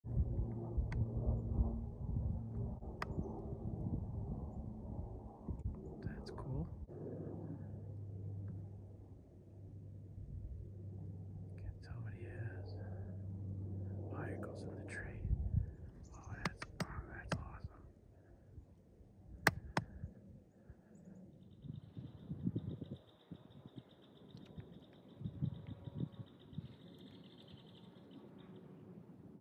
FISHER CAT dinner🐿️
Fierce and ferocious, the elusive Fisher Cat is not often seen... or captured on video, which explains the quality of this video. Even when they're only a short distance away, with only seconds to spare before they disappear, the Fisher Cat does its best at not wanting to be seen. Although the video doesn't accurately capture or portray the size, this Fisher Cat was about 20 pounds of fury. Don't let pictures fool you, they are NOT cuddly animals, rather, a predator's PREDATOR!